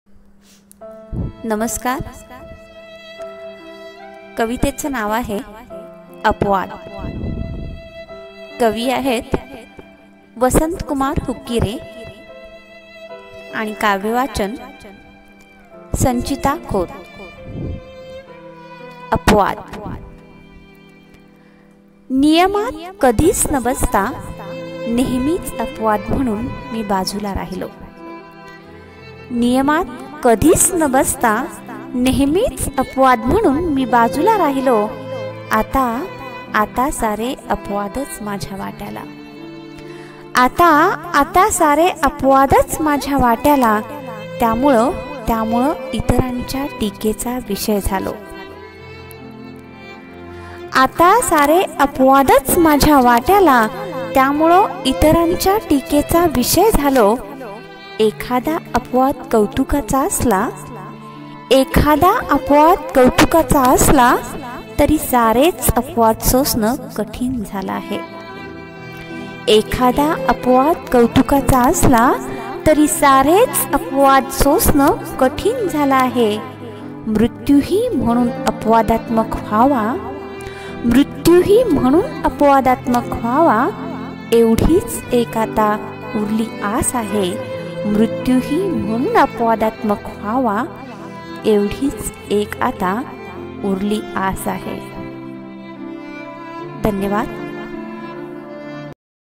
*कवितेचे शीर्षक- अपवाद* कवी - वसंतकुमार हुकीरे,काव्यवाचन- संचिता नितीन खोत (हुकीरे)
चिंतनशील कविता

*कवितेचे शीर्षक-      अपवाद*
 कवी -              *वसंतकुमार हुकीरे*
 काव्यवाचन-    संचिता नितीन खोत (हुकीरे)